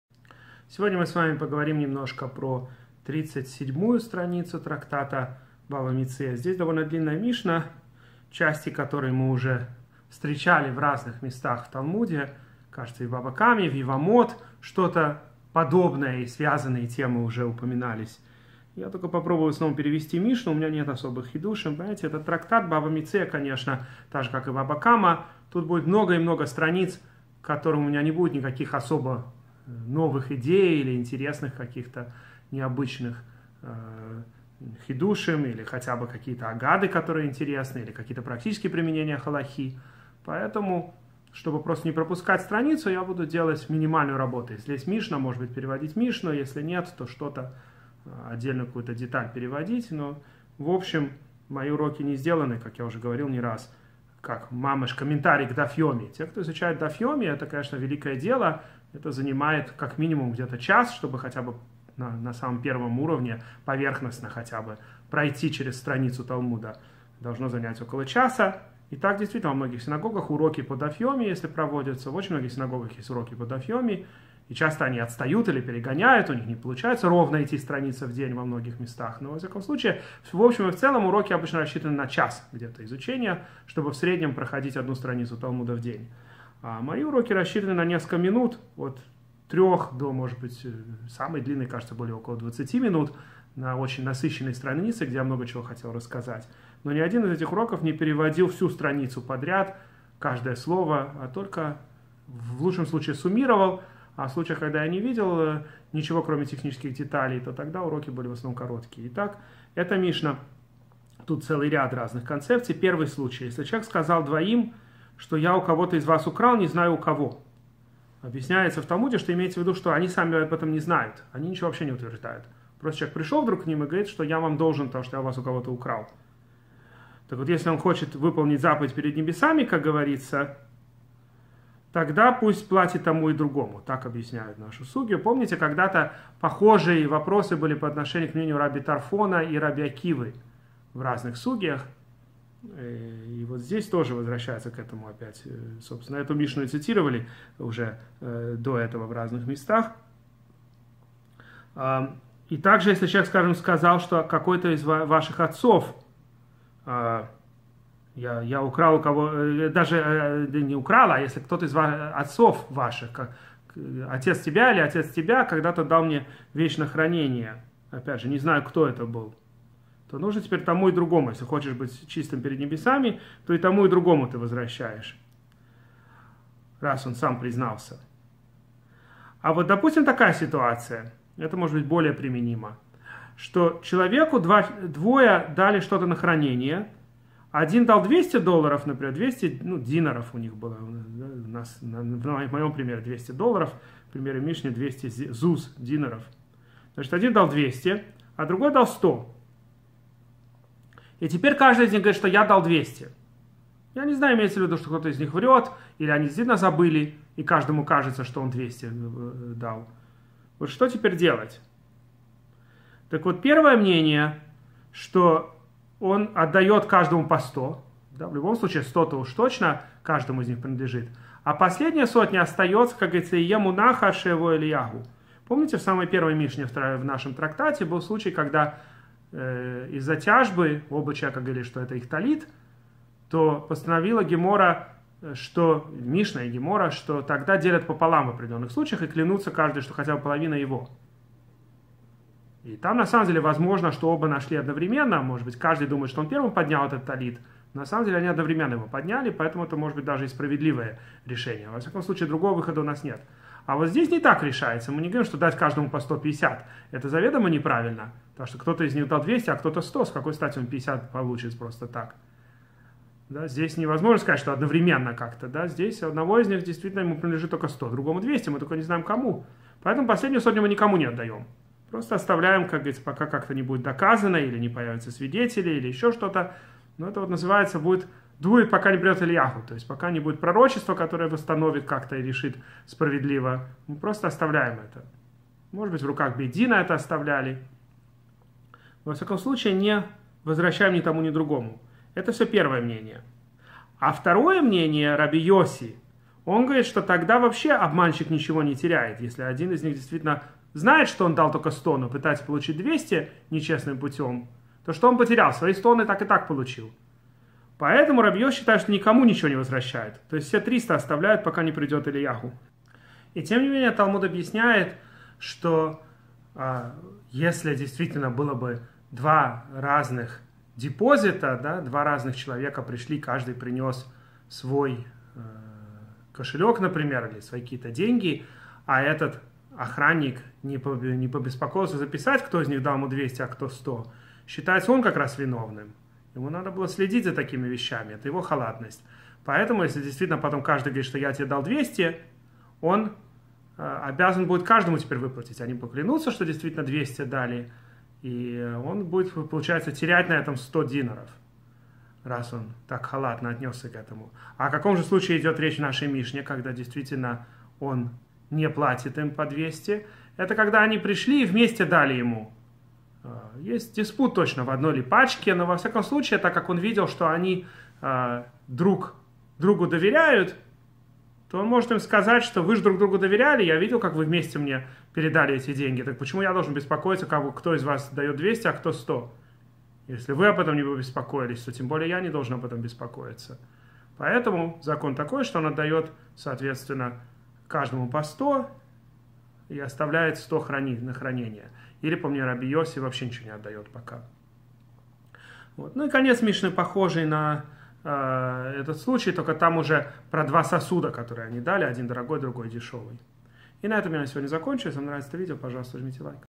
Уроки по Даф Йоми. Бава Мециа. Даф - 37
Что если дали на хранение разные суммы денег, а теперь не известно кто сколько дал?
Урок посвящается для спасения в Израиле, излечения всех раненых и мира.